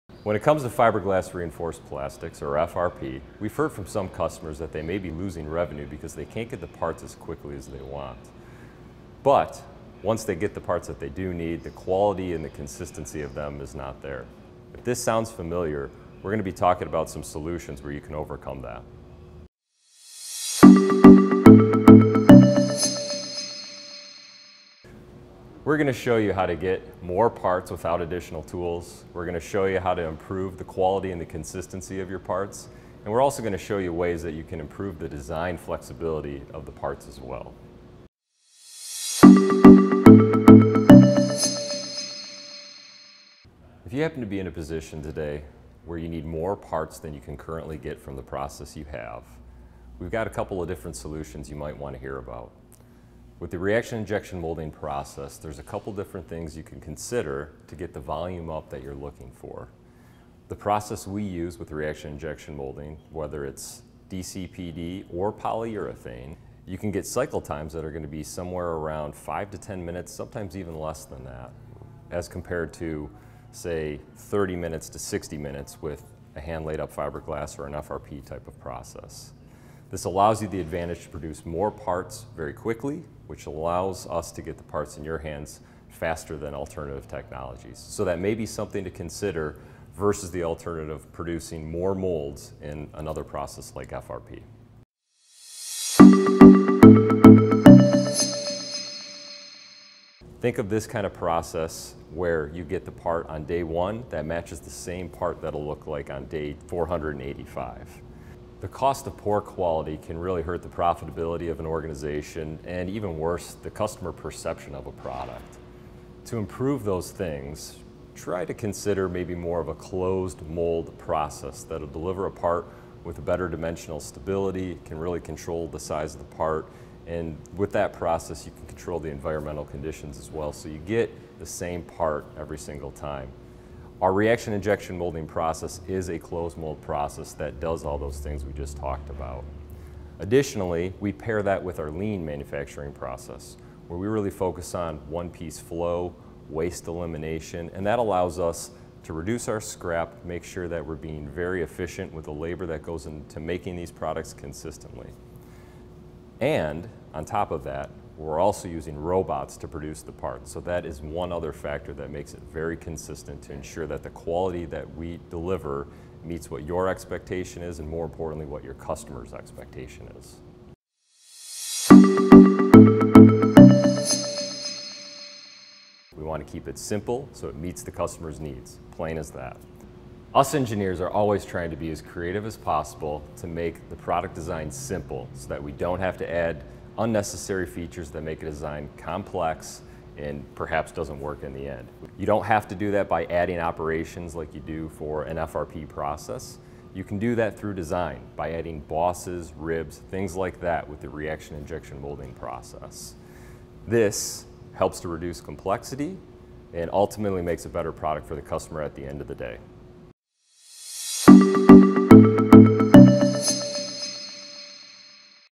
Fiberglass Reinforced Plastics (FRP) - Disadvantages and ways to solve them
Through our collective knowledge of the industry and our close partnerships with our customers, we've been able to identify the 3 most common problems when working with certain composite parts.

- Inconsistent Part Quality
- Limited Design Freedom
- Slow Cycle Times